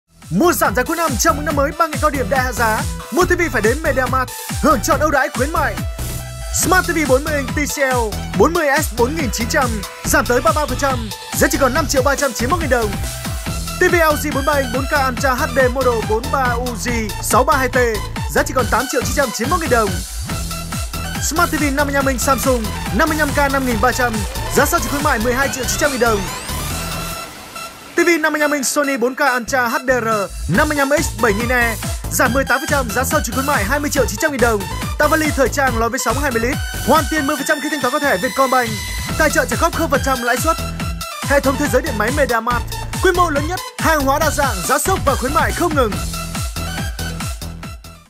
[MEDIAMART] - CAO ĐIỂM ĐẠI HẠ GIÁ TIVI
Với mong muốn gửi tới quý khách hàng những sản phẩm điện máy thông minh nhất, hữu dụng nhất, trong mùa giảm giá cuối năm, MediaMart trân trọng gửi tới Quý khách hàng Chương trình khuyến mại “Chào mừng năm mới – 3 ngày cao điểm đại hạ giá” áp dụng tại tất cả các trung tâm thuộc Hệ thống thế giới điện máy MediaMart trên toàn quốc.

Mở màn là ngành hàng điện tử: Mua TV phải đến MediaMart – hưởng trọn ưu đãi khuyến mại

Tổng đài hỗ trợ miễn phí :
Bán hàng: 19006788
Khiếu nại: 19006741
Bảo hành: 19006743